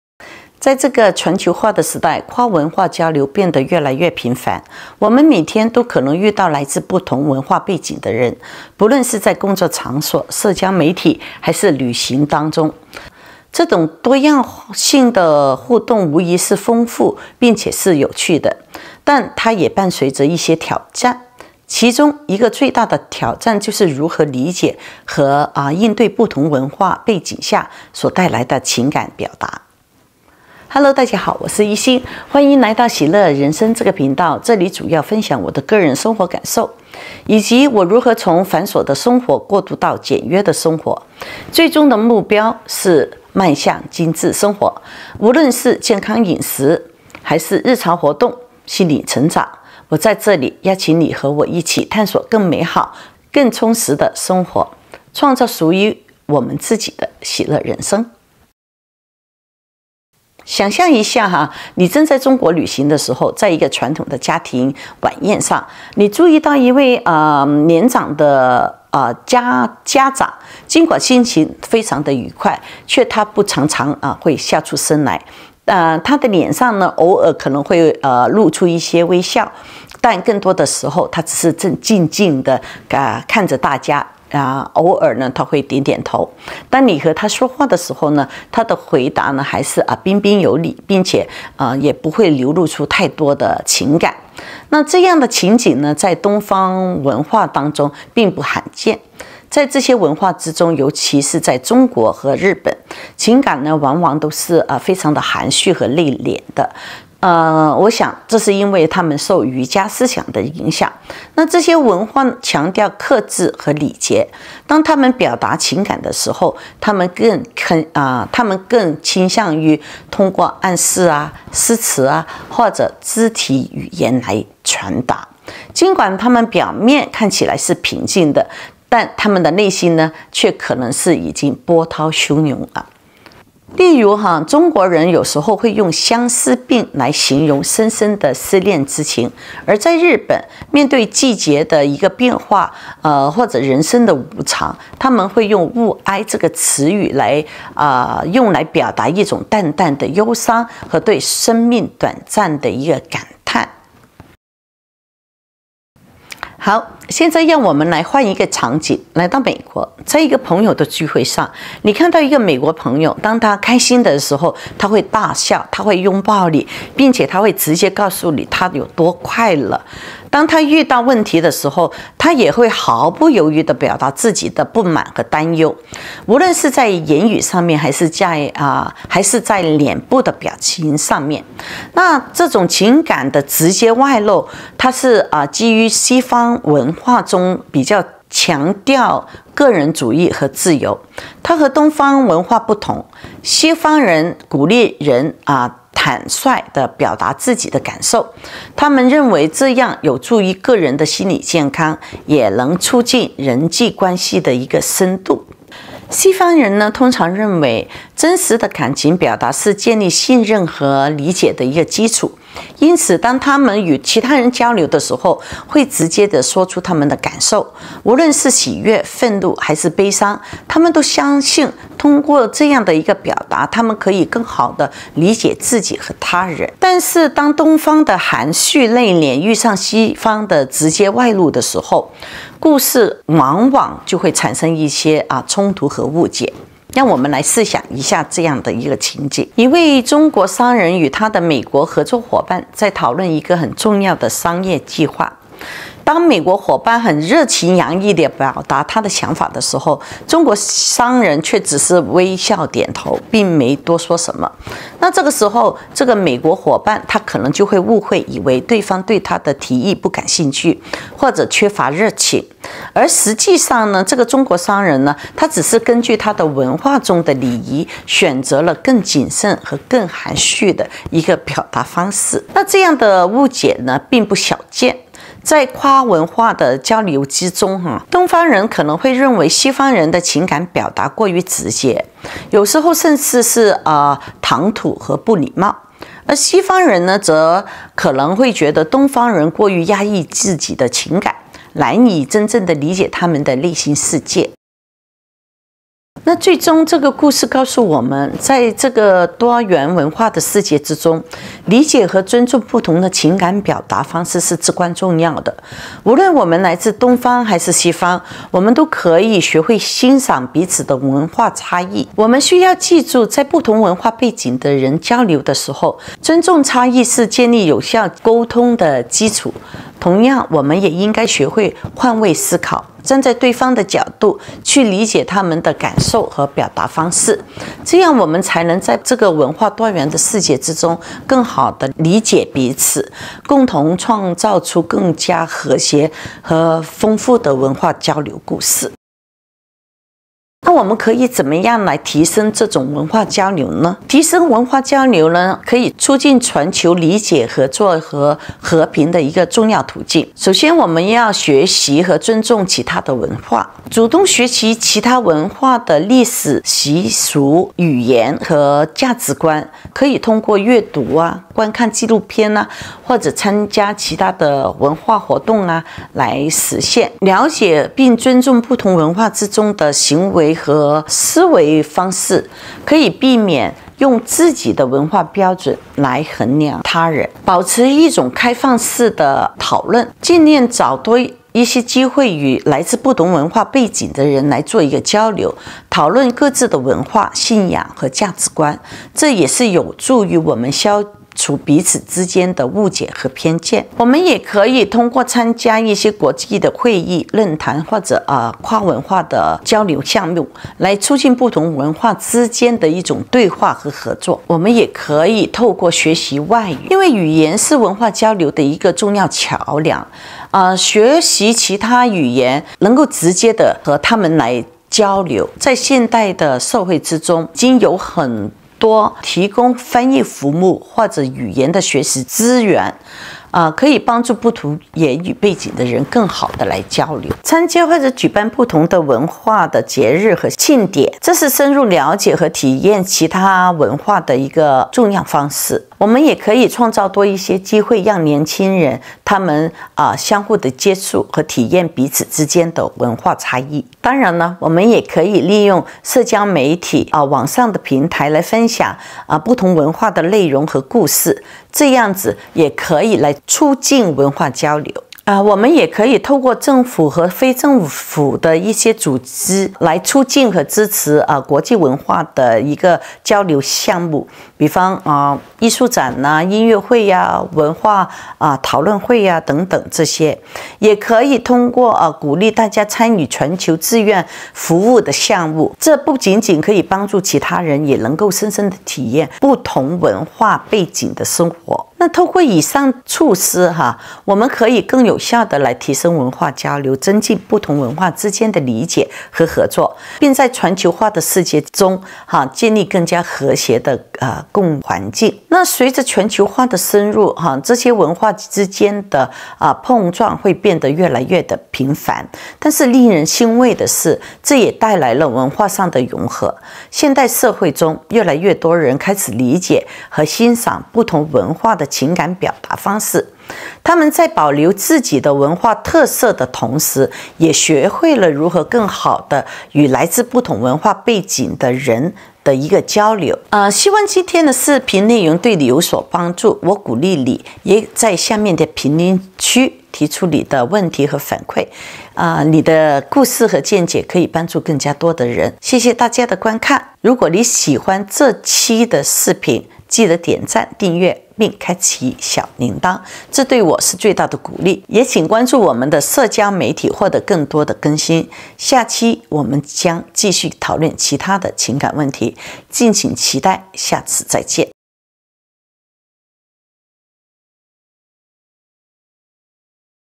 文化碰撞：東西方情感表達大不同
在這個全球化的時代，跨文化交流變得越來越頻繁。我們每天都可能遇到來自不同文化背景的人，不論是在工作場所、社交媒體，還是旅行途中。這種多樣性的互動無疑是豐富且有趣的，但它也伴隨著挑戰——其中一個最大的挑戰就是如何理解和應對不同文化背景下的情感表達。